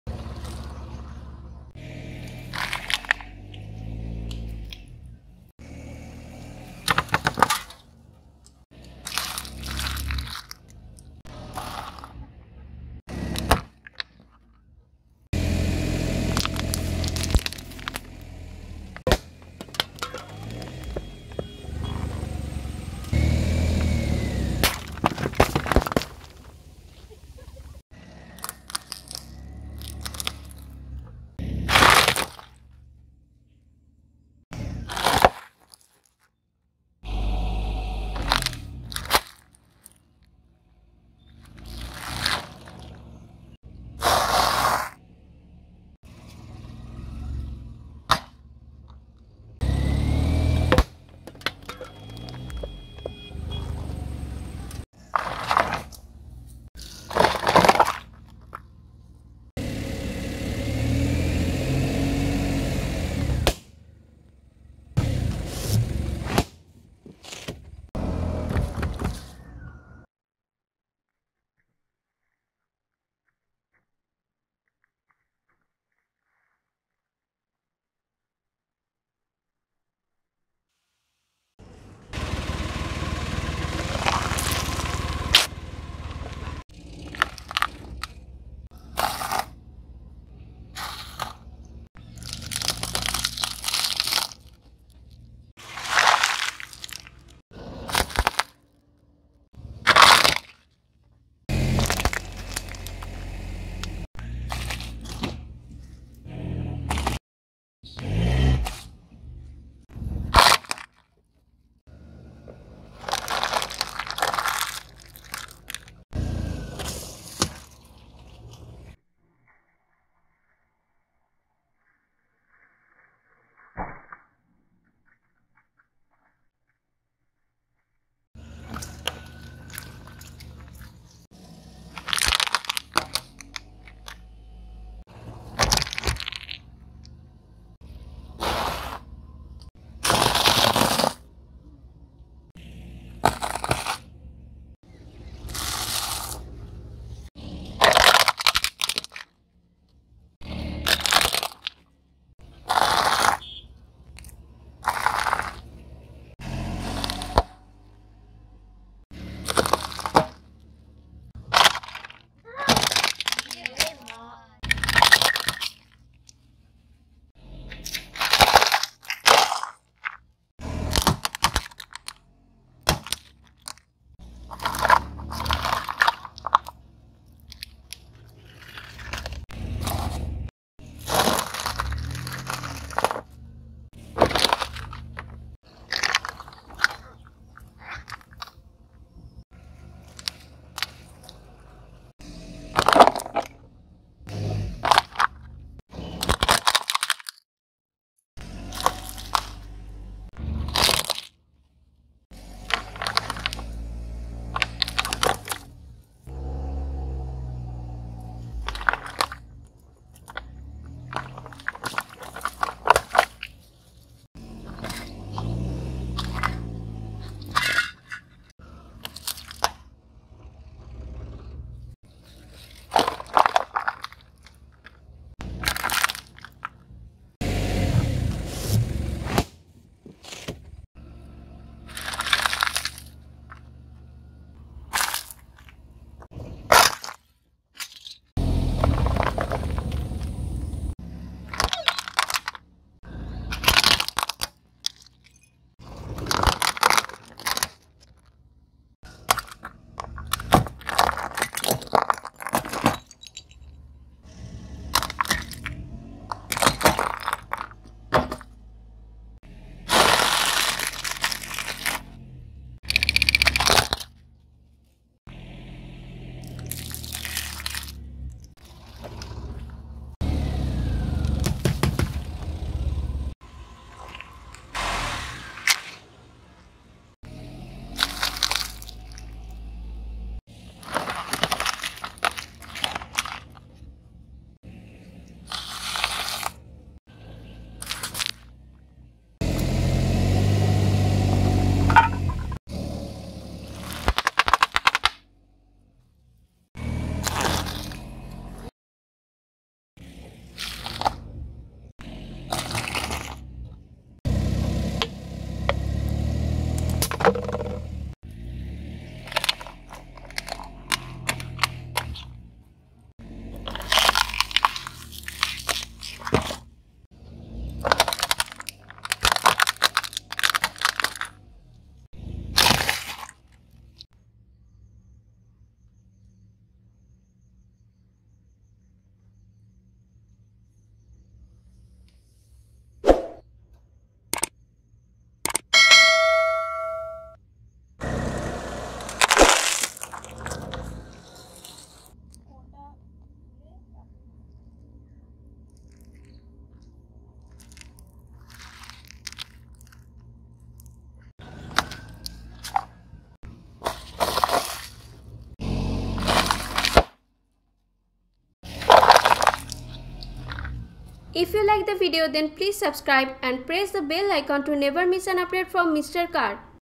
Crushing Crunchy & Soft Things by Car! - Experiment Car vs Ice Cream Candy Cone.
Crushing Crunchy & Soft Things by Car Experiment!